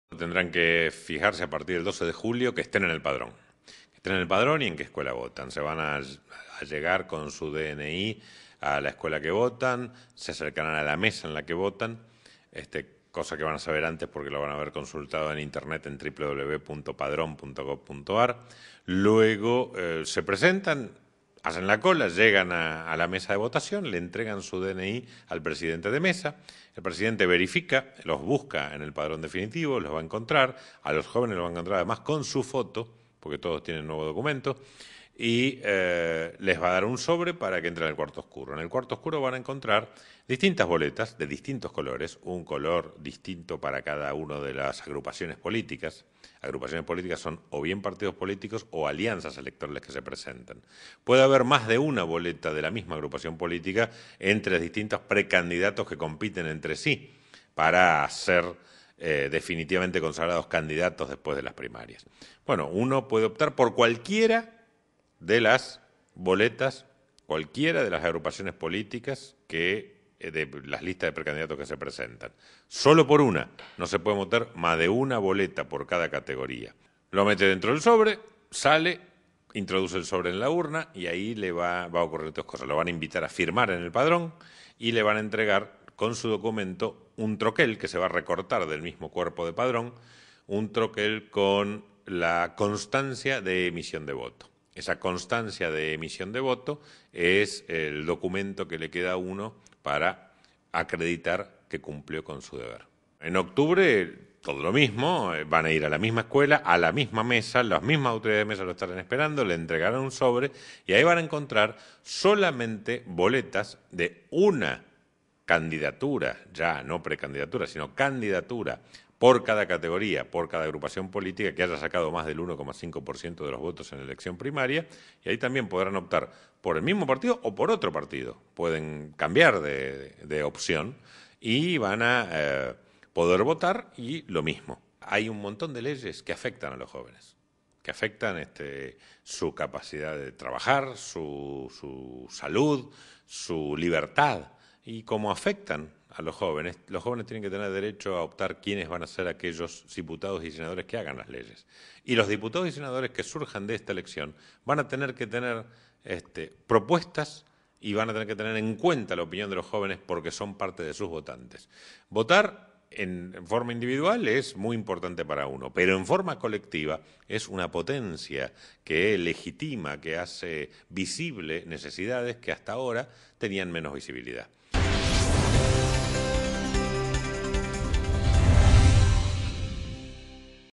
Tullio: "Quienes resulten elegidos en octubre deberán tener muy en cuenta la opinión de los jóvenes"
El Director Nacional Electoral Alejandro Tullio sostuvo hoy que los jóvenes que votarán por primera vez en las próximas elecciones primarias y parlamentarias de agosto y octubre respectivamente, pasarán a ser verdaderos sujetos de derecho por primera vez en la historia.

Tullio además brindó una didáctica explicación acerca de cómo tienen que comportarse en la mesa electoral y en el cuarto oscuro para todos los jóvenes a partir de 16 años que por primera vez emitirán su voto.